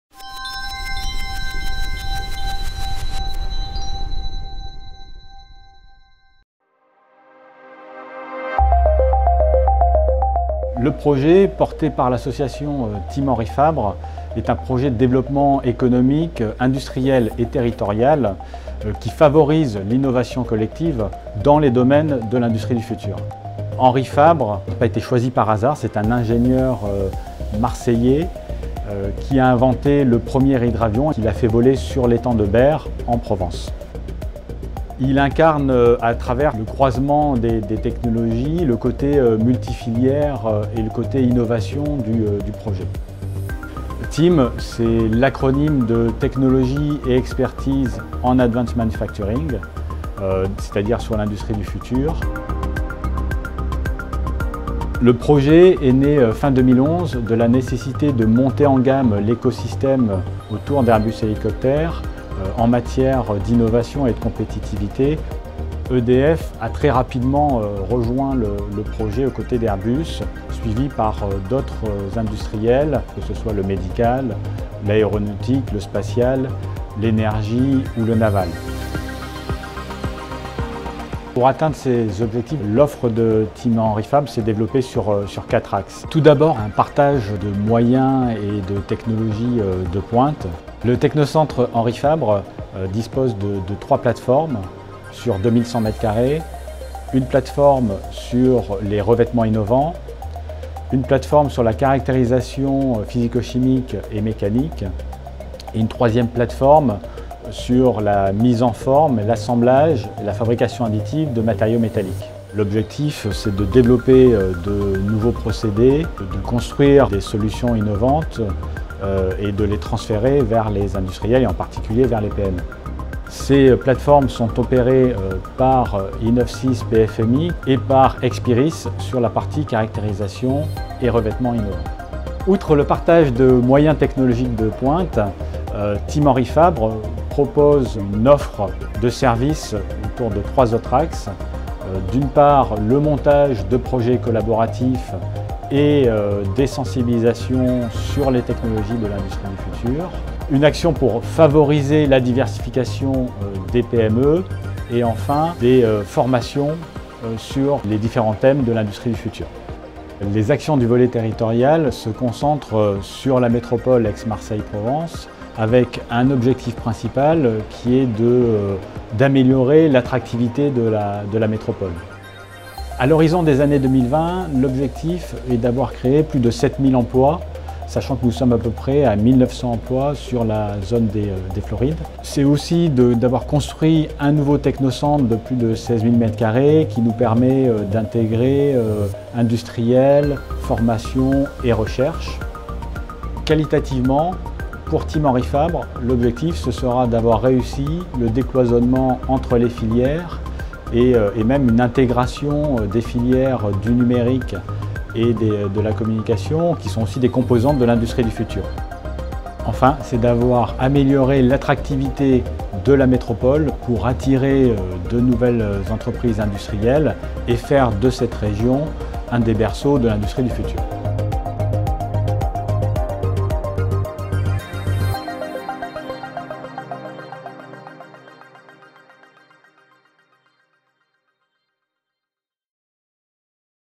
FILM Technocentre Henri Fabre 2016
Retour sur ...
Industriels, élus, représentants de l’État, acteurs académiques... étaient tous ce 23 septembre 2016 sur le Technoparc des Florides à Marignane pour inaugurer le Technocentre Henri-Fabre, la pièce centrale du projet de l’industrie du futur en Provence.